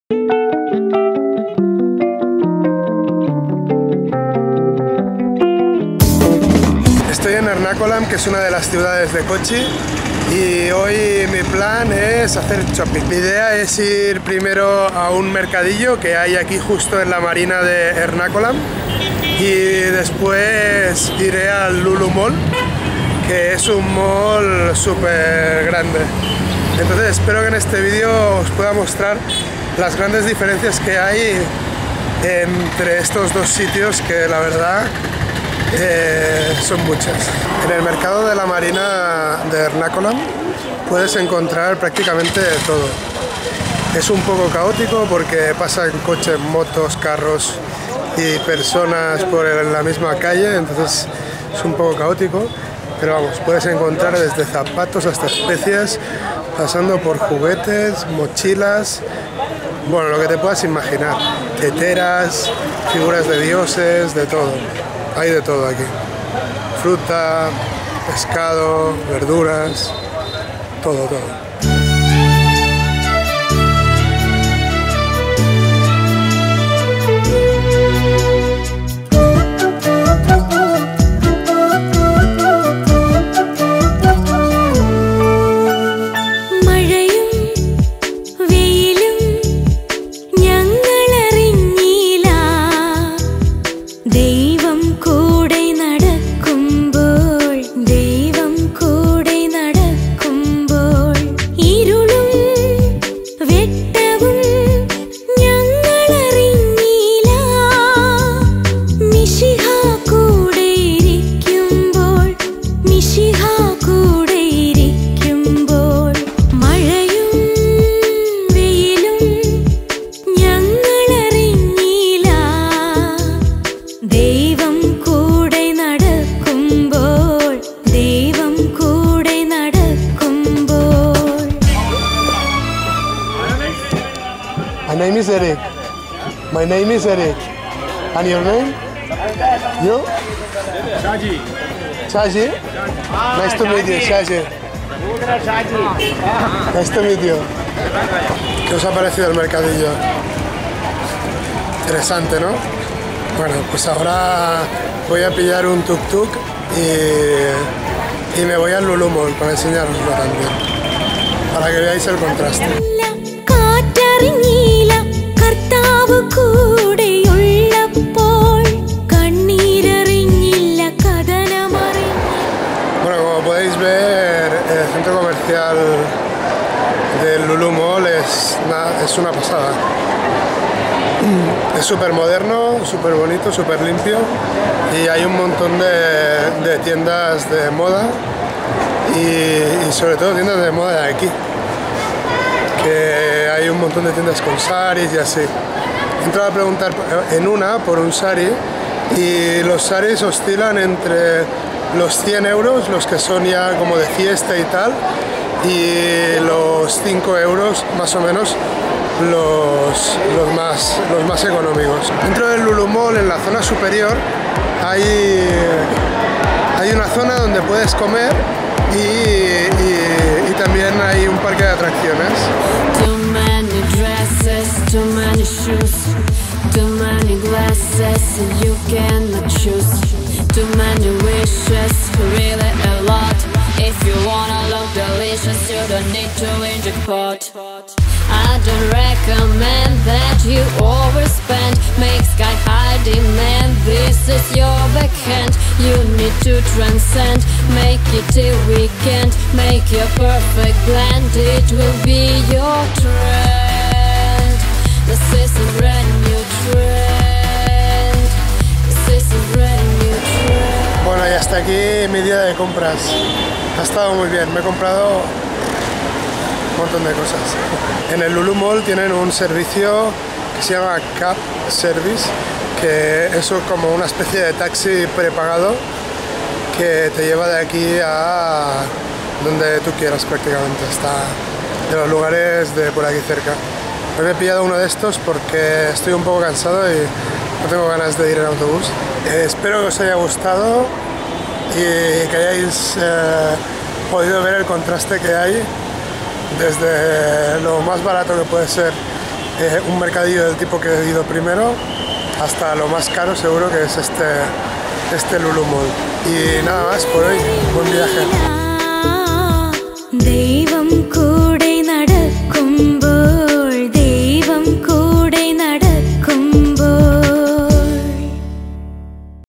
Mercadillo Indio contra Mall de Lujo Indio ... Compras en cochin [Viajar al sur de la India]
En este video os muestro el contraste de comprar en un mercadillo de cochi (el marine drive market) y comprar en una centro comercial de cochi (Lulu mall).